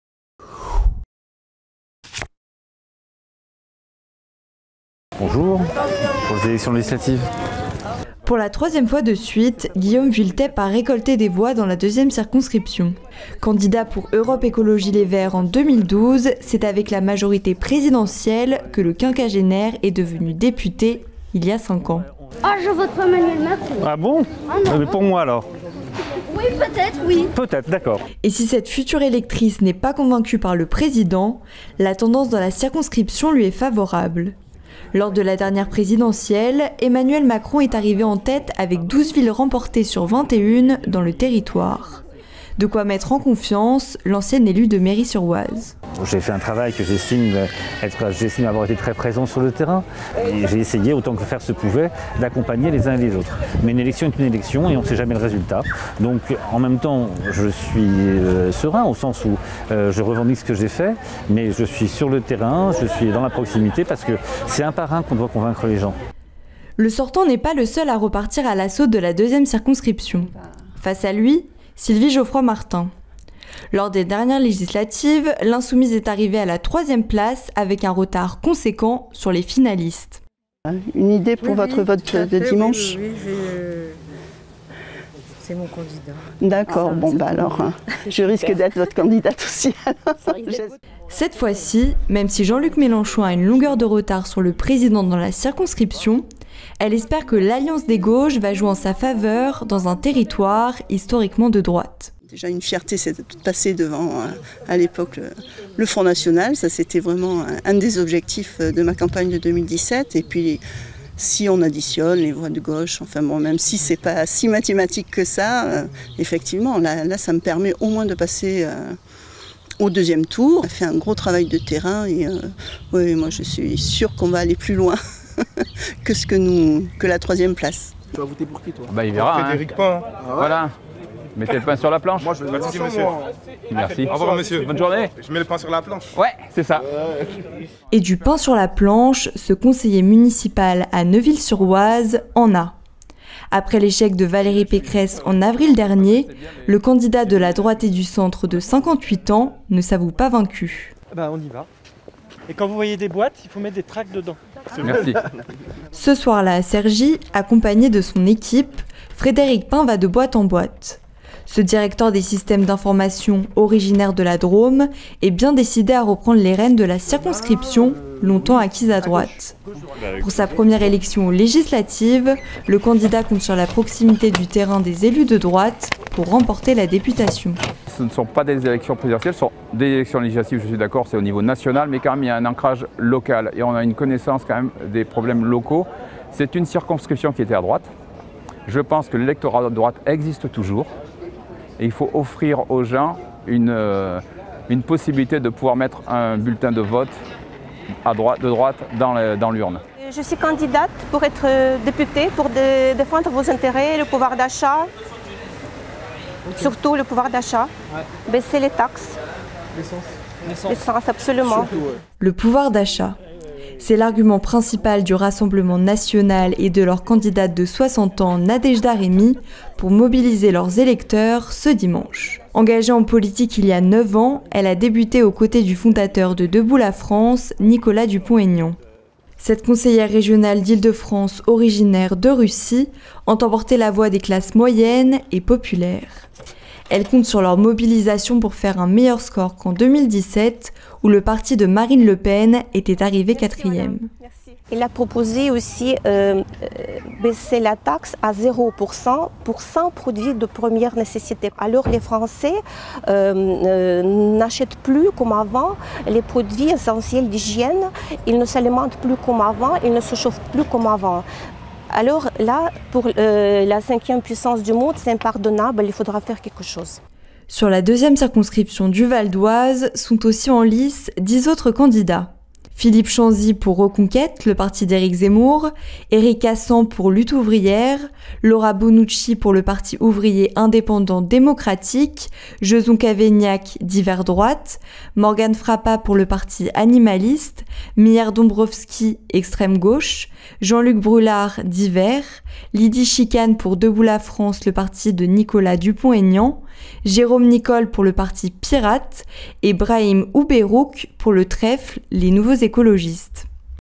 2e circo : Guillaume Vuilletet face à LR et à l’alliance des partis de gauche
Guillaume Vuilletet brigue un second mandat dans la 2e circonscription, historiquement de droite. Ancien écologiste, il avait défait le Républicain Axel Poniatowski en poste depuis 2002. Un siège que le nouveau visage de la droite et du centre, Frédéric Pain, compte bien récupérer. Mais il faudra compter sur Sylvie-Geoffroy Martin qui se représente sous une nouvelle étiquette : NUPES.